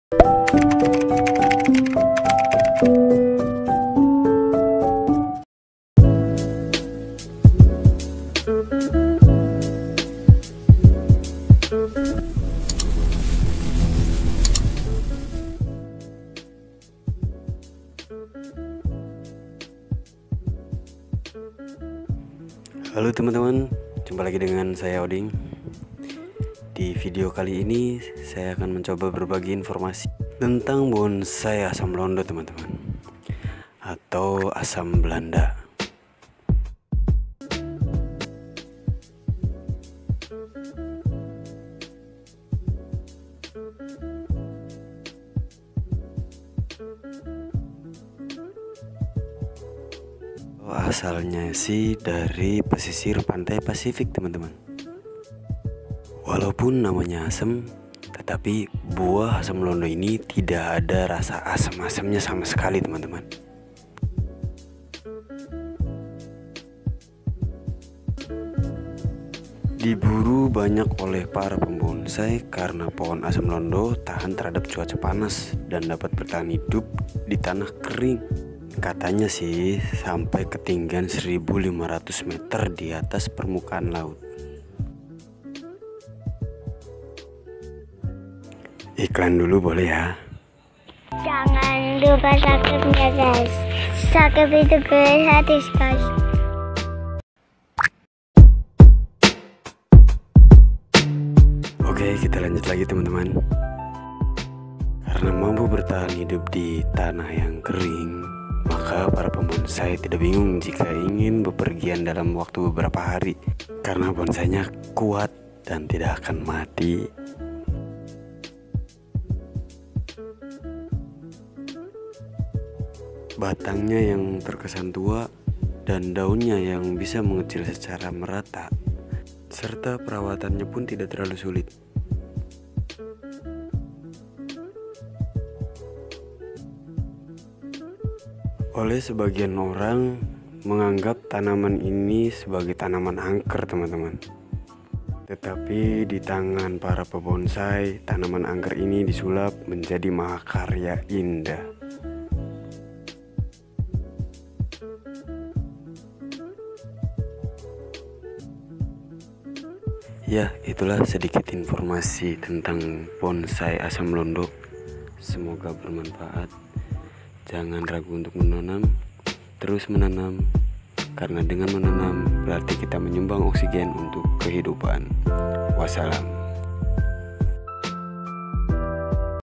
BONSAI ASAM LONDO (PITHECELLOBIUM) || ASAM TAPI T1DAK ADA RASA ASAM ASAMNYA SAMA SEKALI
asam londo sering kali oleh sebagian orang digunakan untuk peneduh dipinggiran jalan, bahkan ada yang menggapnya sebagai tanaman yang angker.
tetapi oleh tangan para pembonsai tanaman tersebut dijadikan sebuah karya seni rekayasa tanaman hias yang bernilai baik dari segi estetika keindahan sampai bernilai ekonomis yang tinggi.

semoga bermanfaat,
jangan ragu untuk menanam
terus menanam
dengan menanam
berarti kita menyumbang oksigen
untuk kehidupan